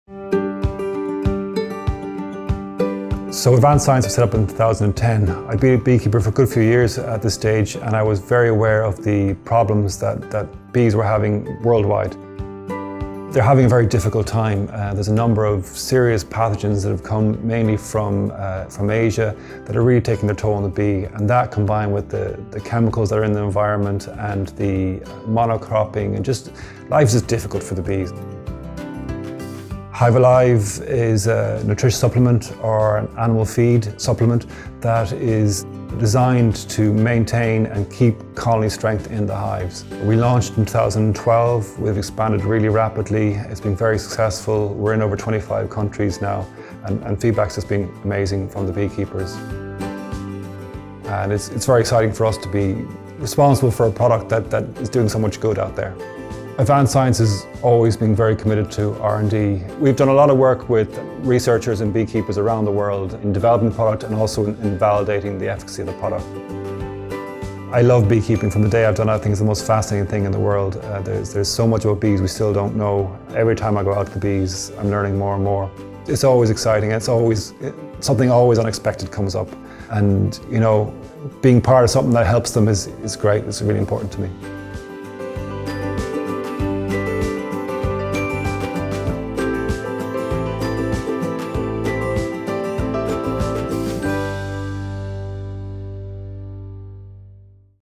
A look into HiveAlive
Take a look into Advance Science. Advance Science produced HiveAlive, a complementary feed for bees. Learn more about HiveAlive here: